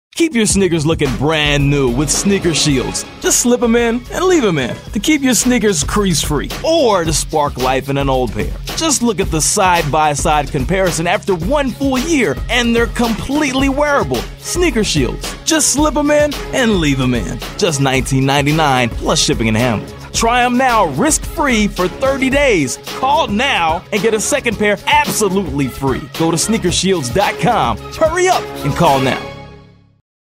Sneaker Shields! As Seen on TV
Protect all your Nike Air Force 1, Jordans, Dunks, and all your casual kicks looking brand new.